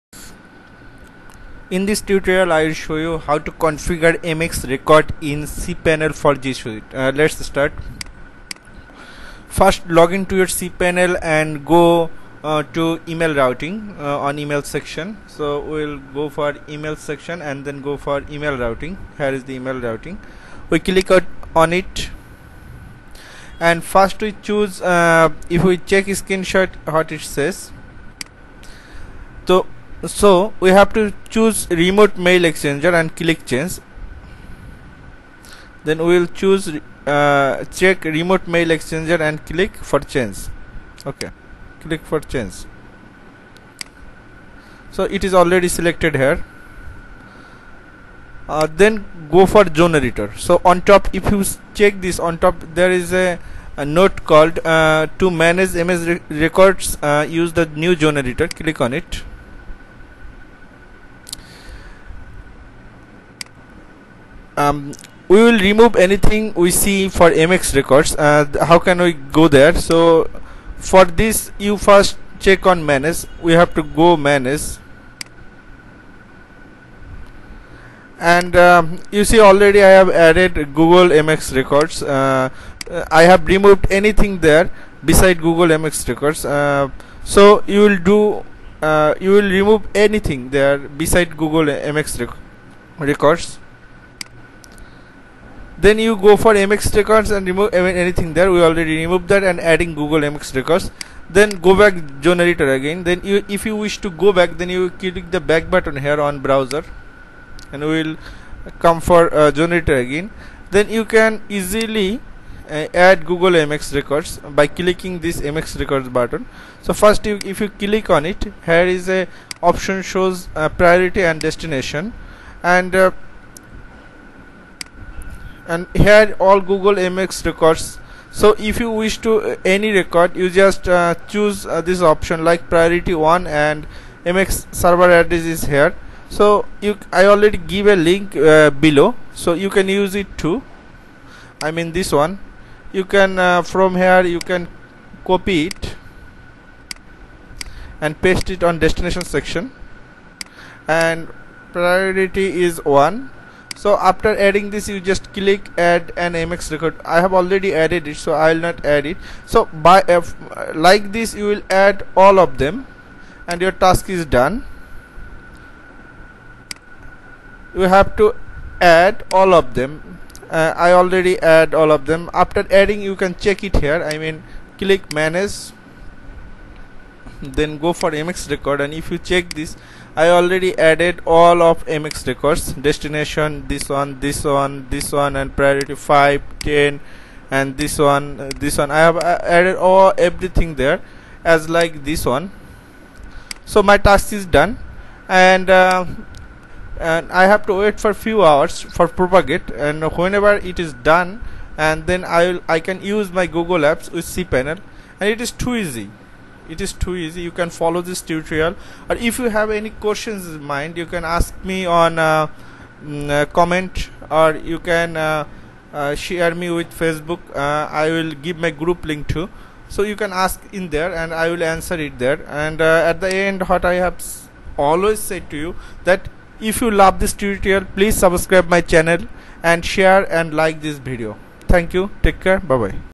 Tutorial 13 - How to configure MX record in cPanel for G Suite
In this tutorial I will show you -  How to configure MX record in cPanel for G Suite